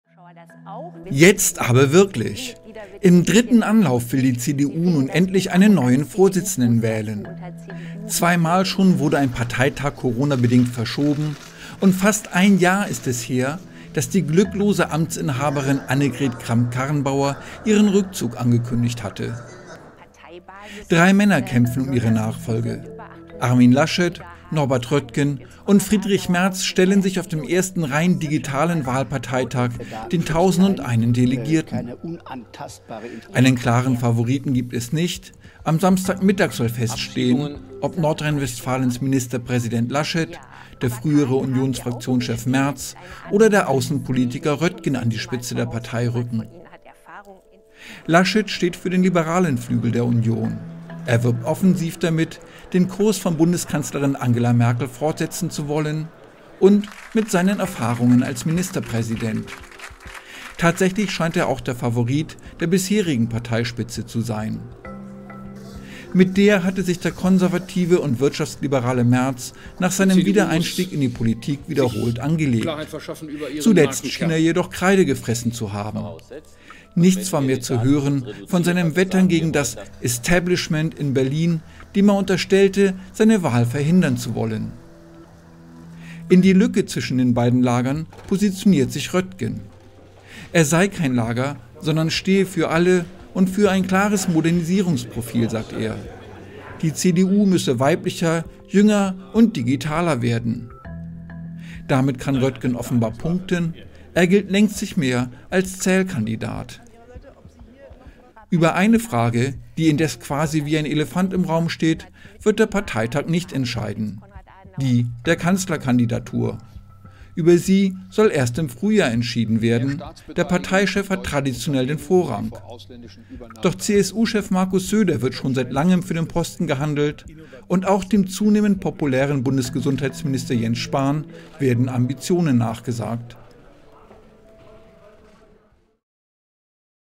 Die CDU auf Chefsuche
Fast ein Jahr ist her, dass Annegret Kramp-Karrenbauer ihren Rückzug als CDU-Vorsitzende ankündigte. Am kommenden Wochenende wird sich entscheiden, ob Armin Laschet, Friedrich Merz oder Norbert Röttgen die CDU ins Superwahljahr 2021 führen.